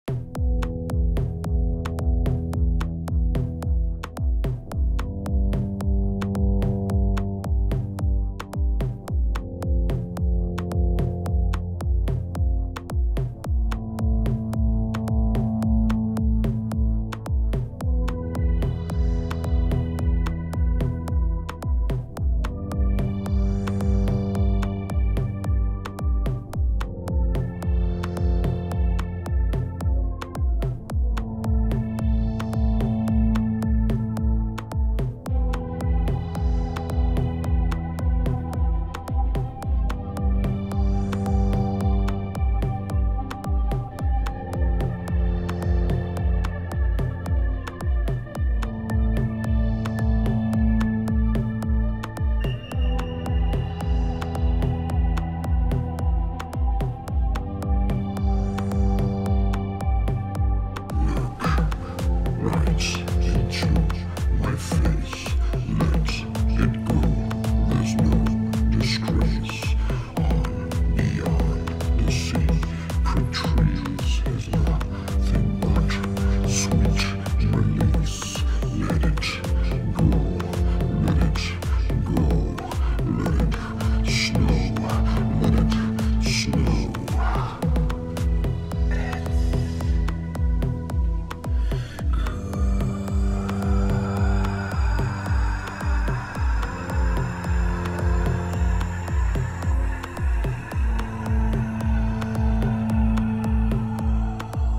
RICEAU Painting poems - Sacred Trees - Trip (digital art, painting, poem, beat, trippy visuals)
A poem created whilst painting a digital painting.
Inspiring the finishing of that painting and the accompanying music in the process.  The painting is the poem and the  poem is the painting.

Poem:
Look right into my face
let it go there's no disgrace
On beyond the sacred trees
Is nothing but sweet release
Let it go let it go
Let it snow let it snow
It's good